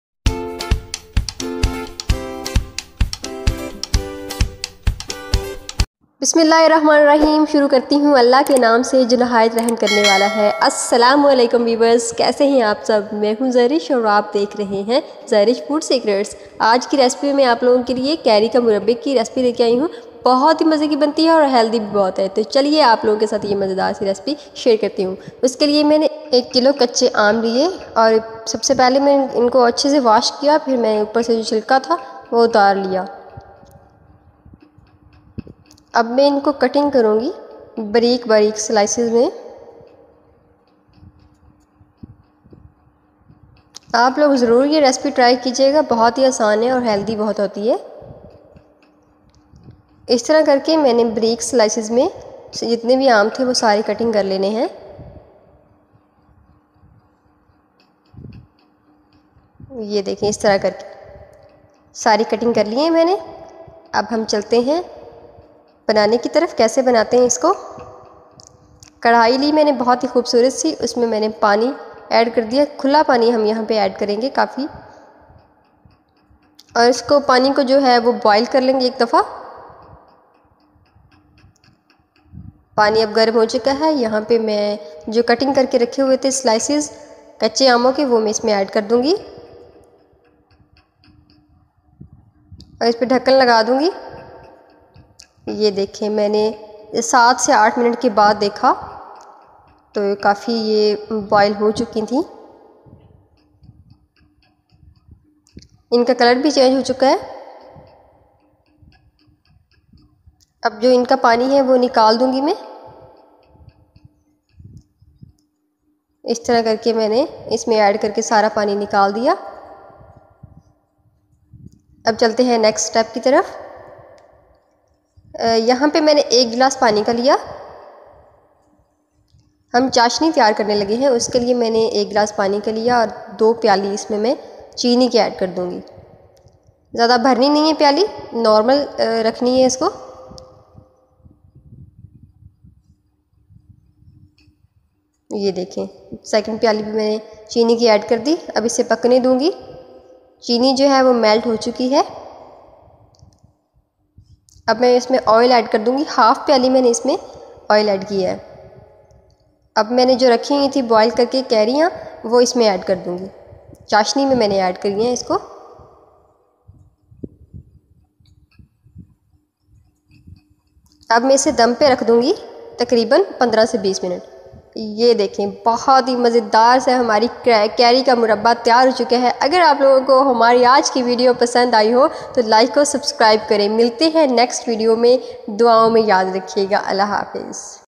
Kachee Amm ka Marba: A Heartwarming Journey of Mother's Love | Zarish Food Secrets | Episode: 88
Witness the beautiful bond between a mother and her child in "Kachee Amm ka Marba." This heartwarming video takes you on an emotional journey, showcasing the unconditional love, sacrifices, and unwavering support of a mother. Get ready to be moved as we delve into the joys and challenges of motherhood, reminding us all of the priceless gift that a mother's love truly is. Don't miss this touching tribute to all the incredible mothers out there!